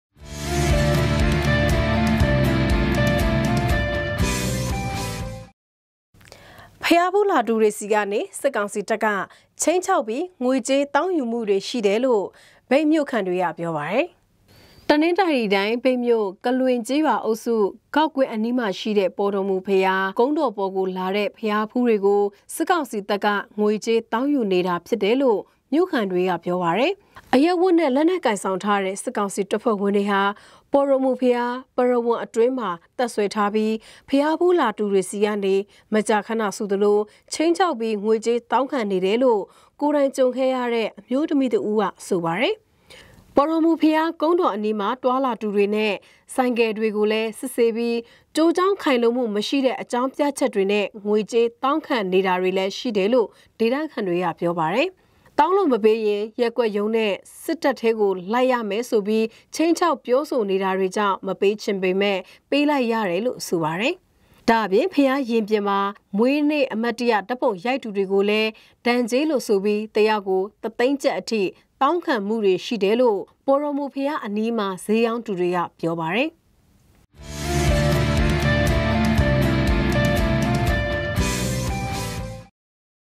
ဘုရားဖူးလာသူတွေကို စစ်ကောင်စီတပ်က ခြိမ်းခြောက်ငွေတောင်း
ဘုရားဖူးလာသူတွေဆီကနေ စစ်ကောင်စီတပ်က ခြိမ်းခြောက်ပြီး ငွေကြေးတောင်းယူမှုတွေရှိနေတယ်လို့ မြိတ်မြို့ခံတွေက ပြောပါတယ်။